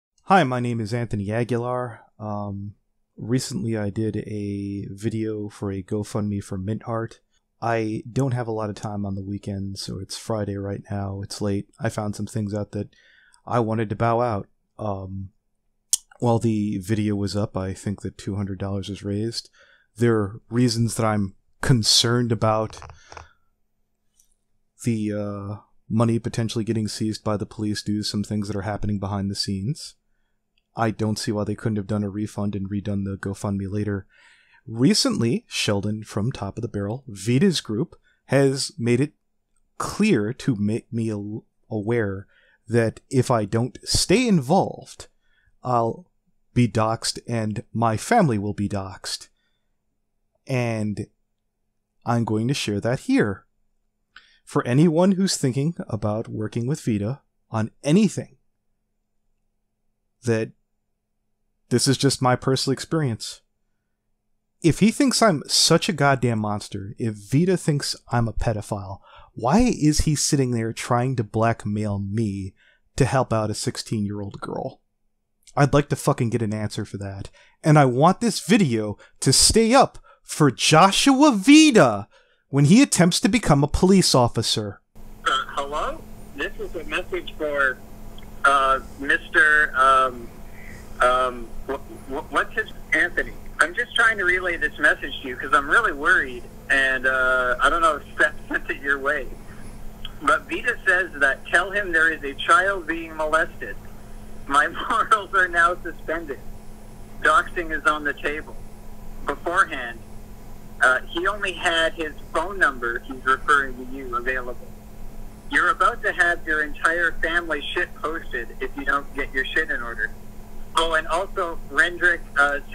vida black mail
I just want out, Mis Anthro pony has 12 thousand
and is close with mint heart.  His play form is twice the size of mine.
I want out.  I want nothing to do with vida or mint heart or any that shit.